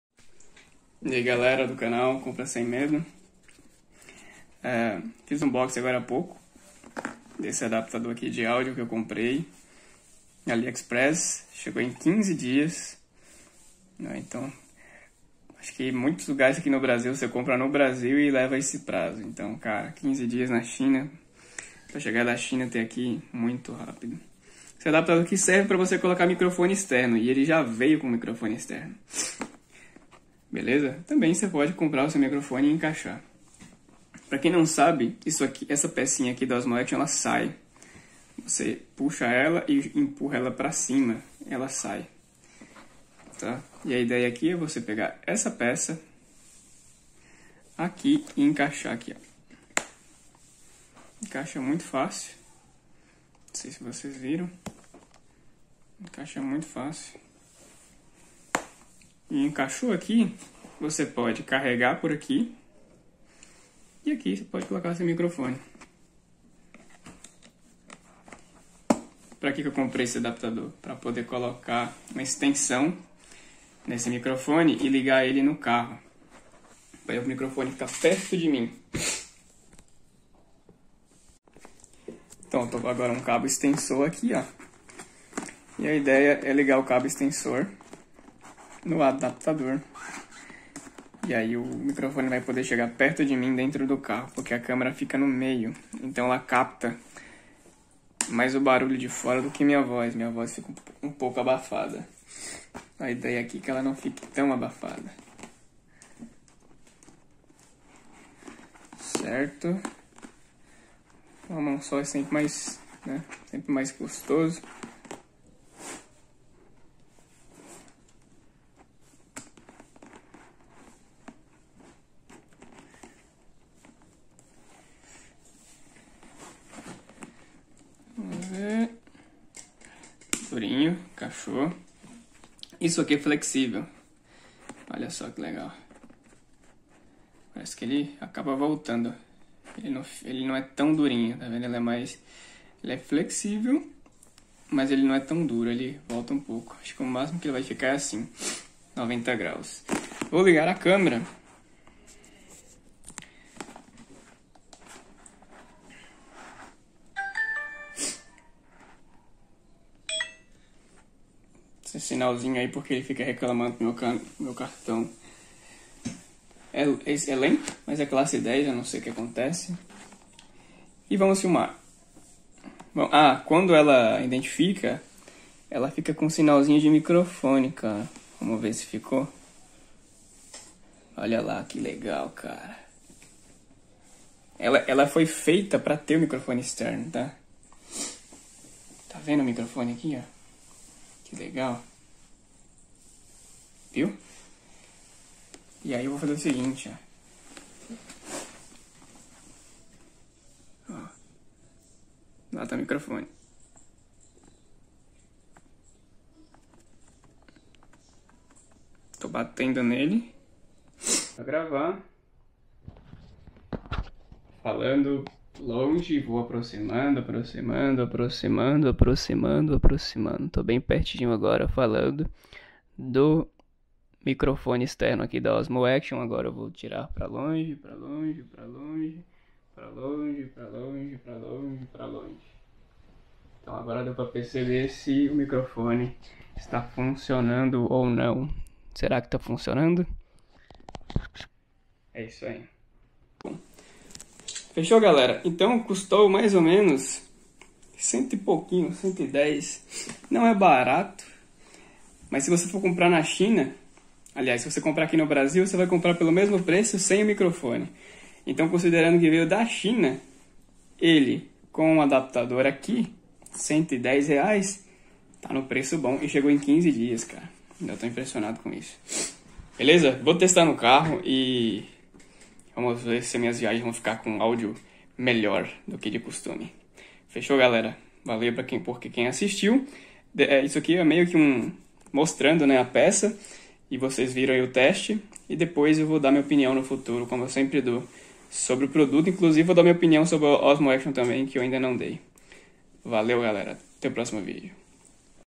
Testando microfone externo câmera DJI Osmo Action
Comprei na loja da DJI no site do Ali Express
Custou R$ 116,00 frete grátis  e veio o adaptador + o microfone externo